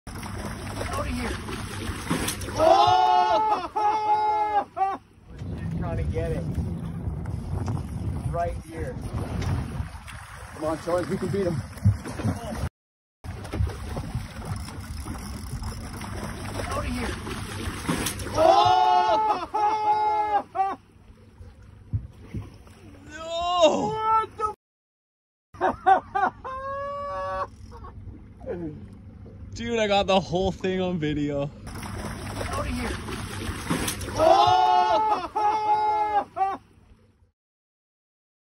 Sea lion steals snack from fisherman's line
A Canadian fisherman wasn’t too upset when a sea lion stole his catch, saying afterwards, he’d just paid a "sea lion tax."